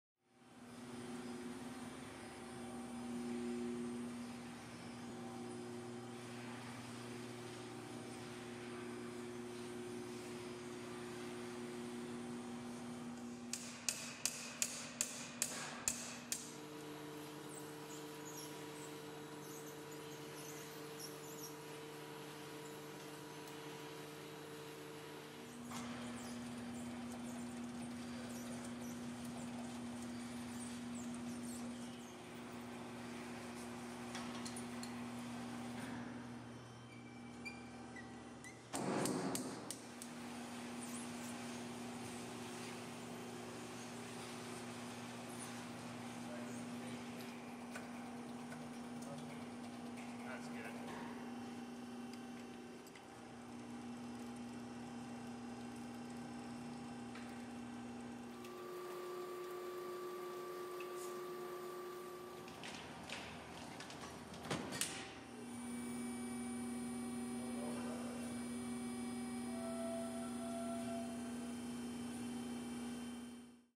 The Shop Dog operating a Lockwood Potato Harvester
See how easily you can operate potato equipment using The Shop Dog.